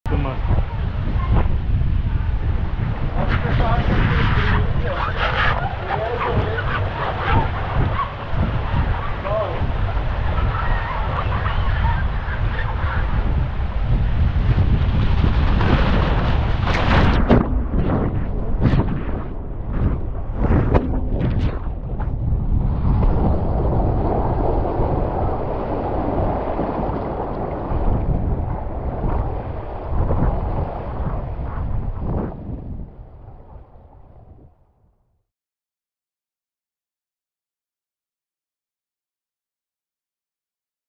Aquapark Nessebar Dragero left Waterslide
POV Video Dragero at Aquapark Nessebar in Bulgaria.